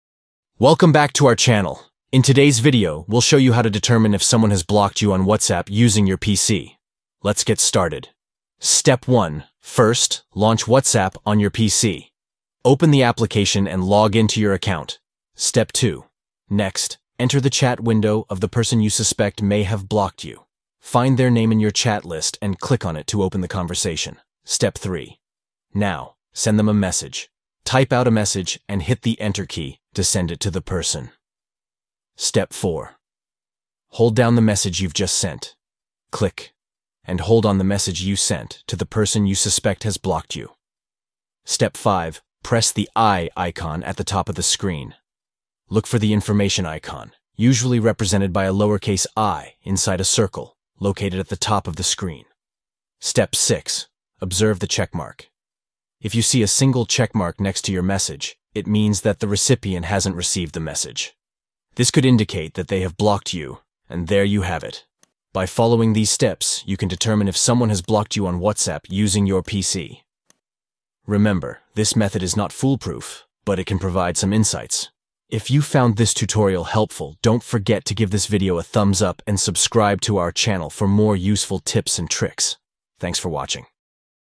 How To Know If Someone Blocked You On WhatsApp On PC
Learn how to find out if someone has blocked you on WhatsApp on your PC in just a few easy steps.

1. Launch WhatsApp on your PC.
2. Enter the chat window of the person you suspect has blocked you.
3. Send them a message.
4. Hold down the message you've just sent.
5. Press the 'i' icon at the top of the screen.
6. Check for a single checkmark to see if the recipient has received the message.

If the recipient has blocked you, the message will not be delivered, and you'll see a single checkmark.
Don't worry, we'll show you how to do this quickly and easily.
Watch now and find out if you've been blocked on WhatsApp.